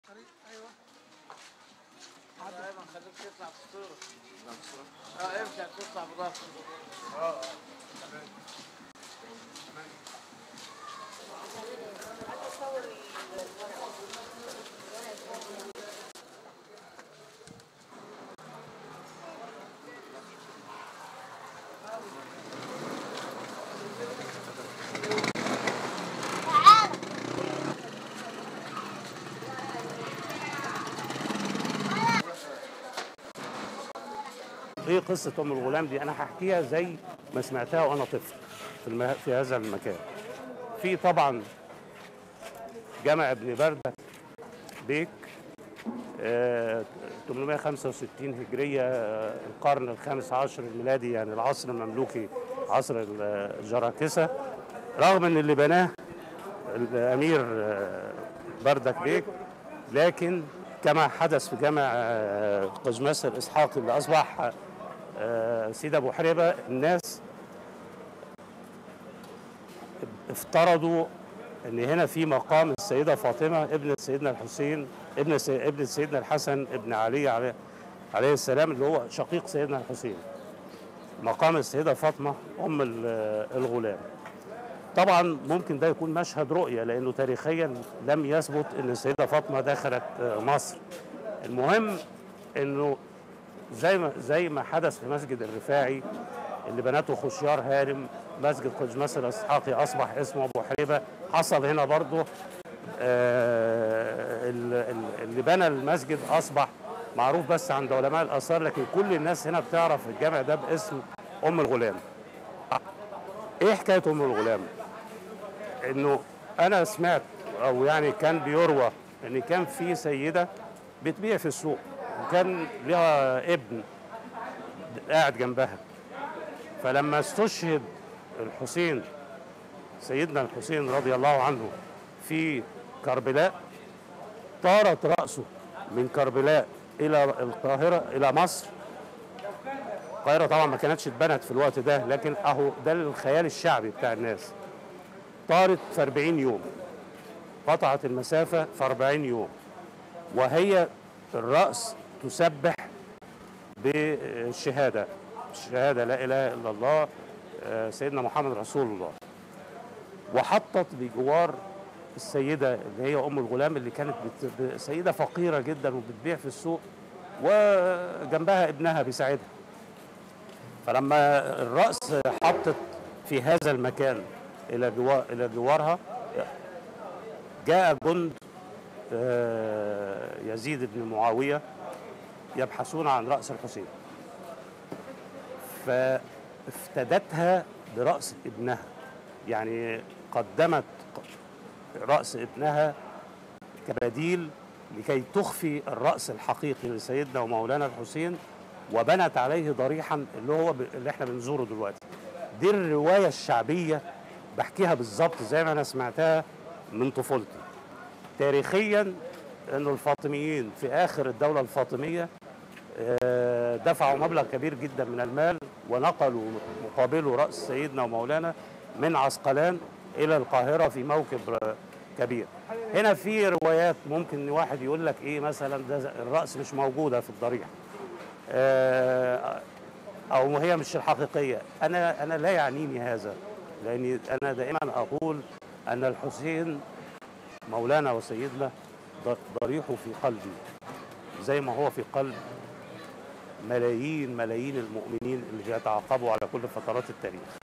تجليات مصرية.. قصة مقام ومشهد السيدة فاطمة أم الغلام بالقاهرة القديمة
3 فبراير 2023